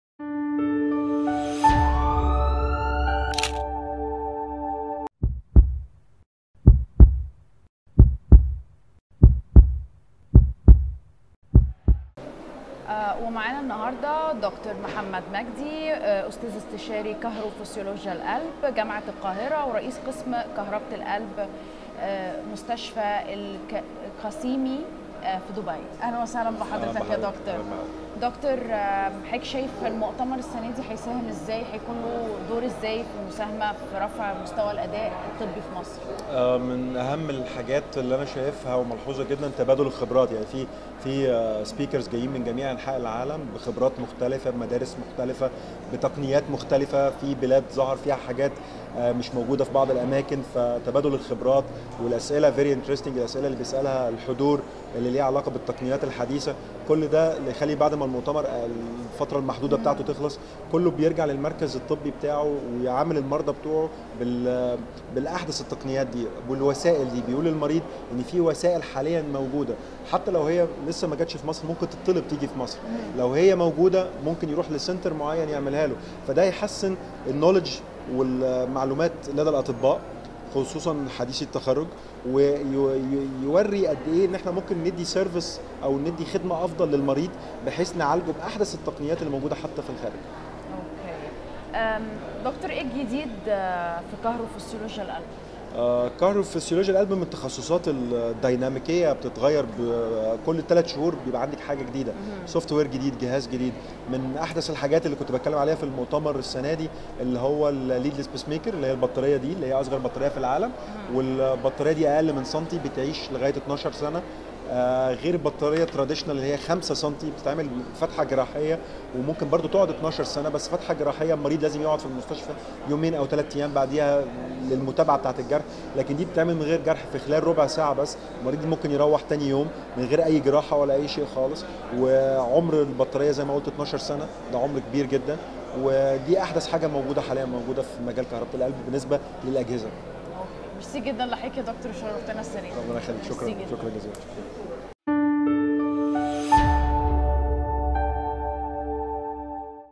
الدكتور محمد مجدى يتحدث عن مساهمة المؤتمر فى رفع المستوى الطبى فى مصر
الدكتور محمد مجدى
استاذ استشارى كهروفسيولوجى القلب جامعة القاهرة
رئبس قسم كهربة القلب بمستشفى الفسيمى دبى
يتحدث عن مساهمة المؤتمر فى رفع المستوى الطبى فى مصر